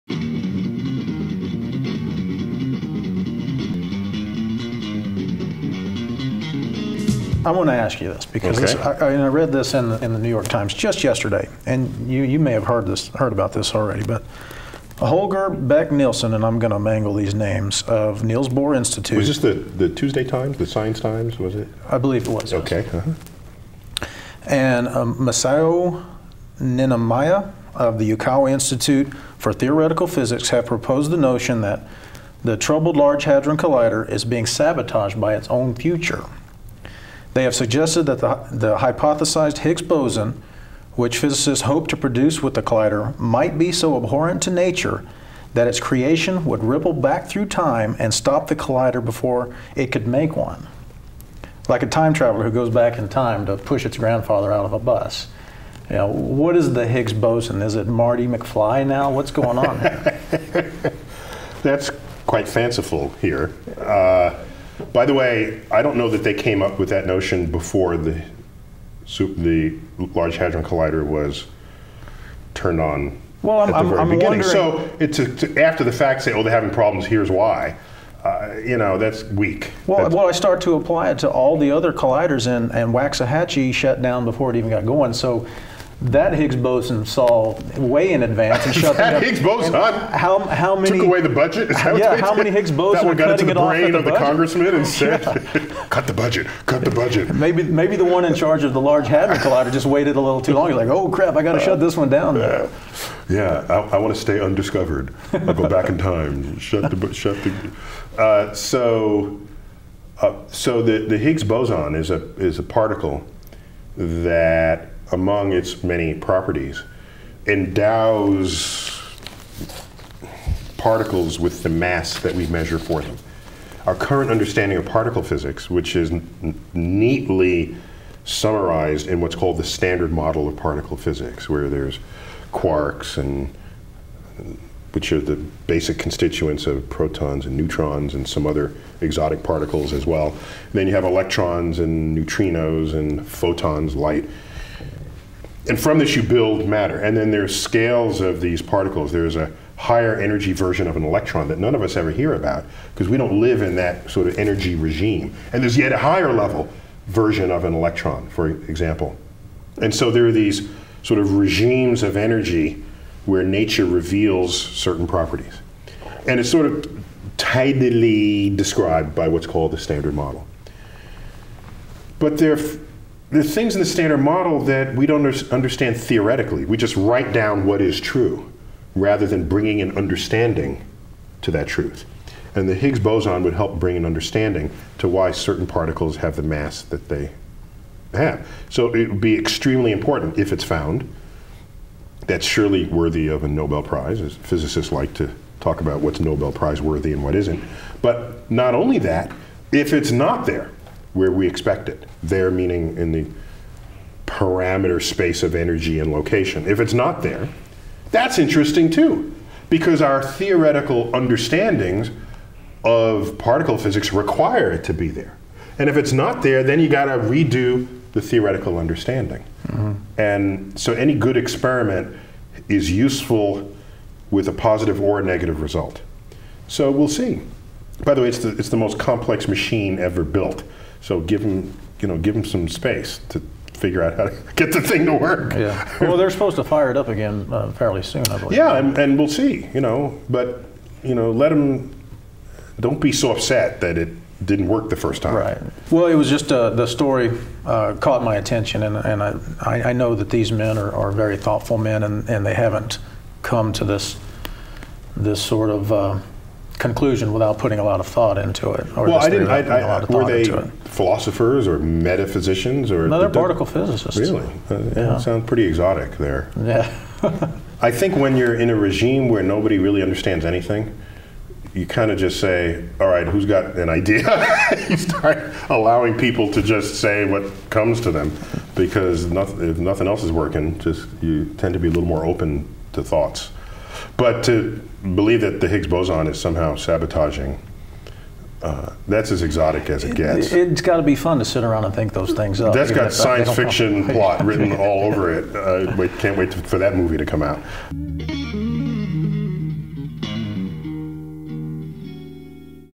Skepchick Presents: A Conversation with Dr. Neil deGrasse Tyson (Part 4)
Sam Ogden, contributing writer for the wildly popular skeptical blog, Skepchick, sits down with Dr. Neil deGrasse Tyson of the Hayden Planetarium and the NOVA scienceNOW program for a conversation about the universe, scientific literacy, and sundry other topics.